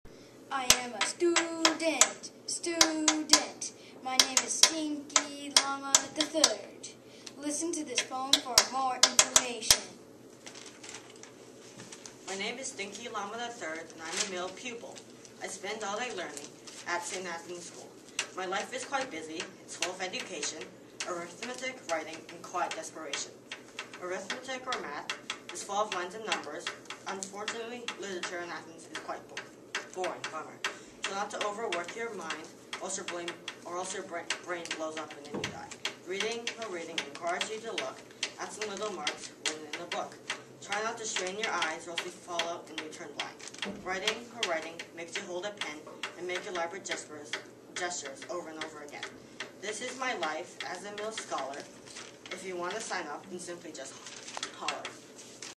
Life in Ancient Athens Song
Watch students perform original songs about daily life in Ancient Athens.